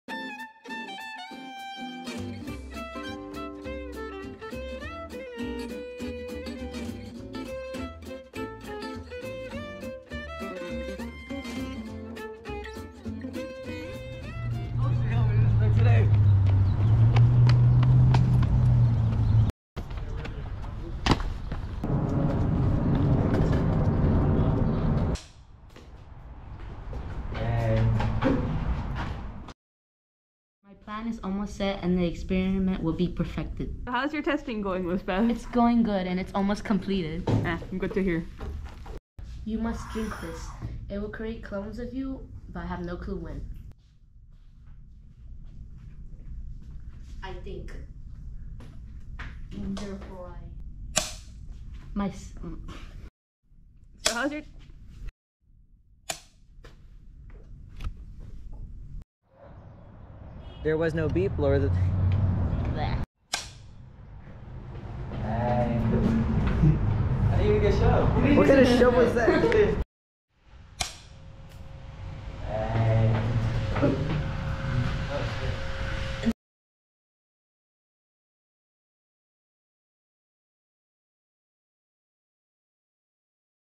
Film name: clones of good ol Isaac (Not a Minecraft video)
this is a short film me and a group of classmates made for our film class final project.